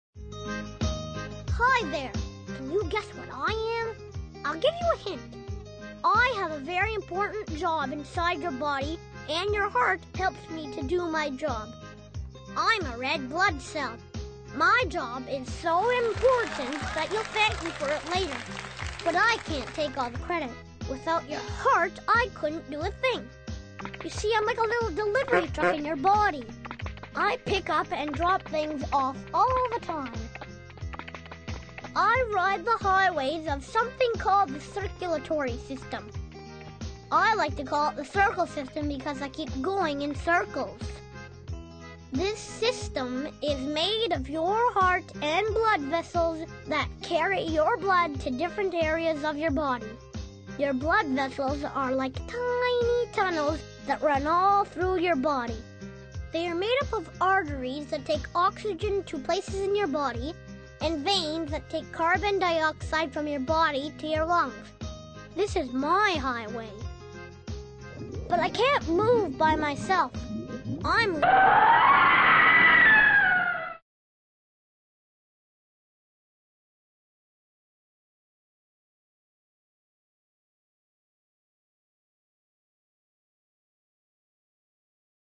Circulatory System for Kids
The circulatory system, also called the cardiovascular system, is an organ system that permits blood to circulate and transport nutrients (such as amino acids and electrolytes), oxygen, carbon dioxide, hormones, and blood cells to and from cells in the body to nourish it and help to fight diseases, stabilize body...